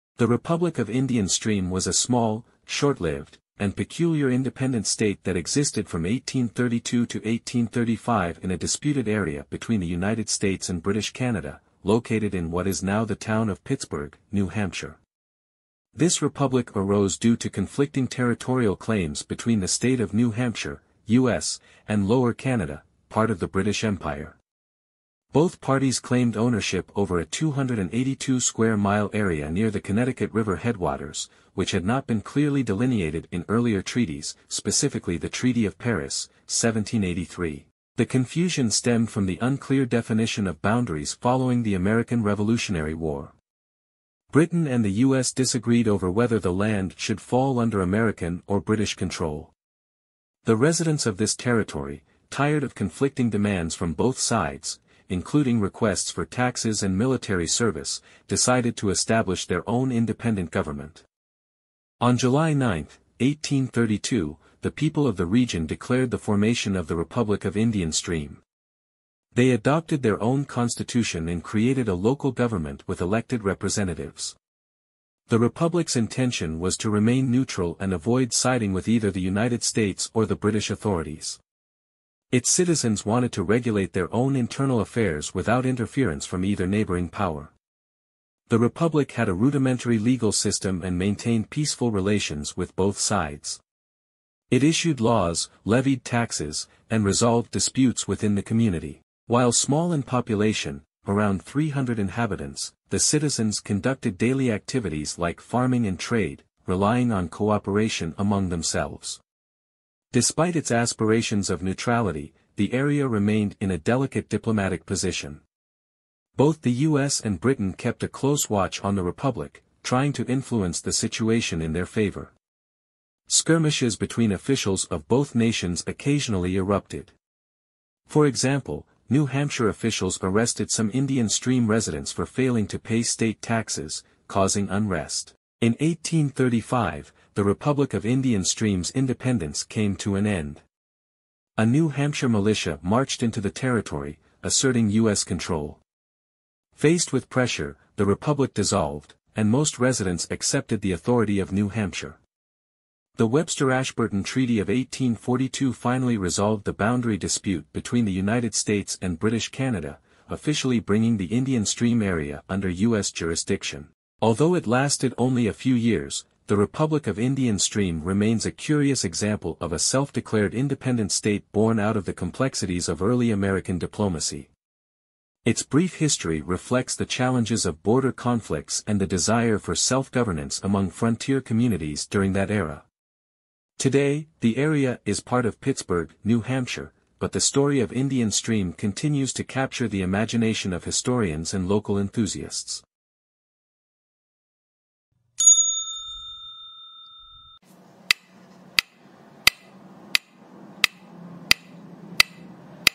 The Republic of Indian Stream | Short-Lived Independent State (1832-1835)
The Republic of Indian Stream was a unique, short-lived independent state that existed from 1832 to 1835 in a disputed border region between the United States and British Canada. Located in what is now Pittsburg, New Hampshire, this small republic emerged from territorial conflicts between the state of New Hampshire (U.S.) and Lower Canada (part of the British Empire). Both sides laid claim to a 282-square-mile area near the Connecticut River headwaters, where the boundaries had not been properly defined by previous agreements, including the Treaty of Paris (1783).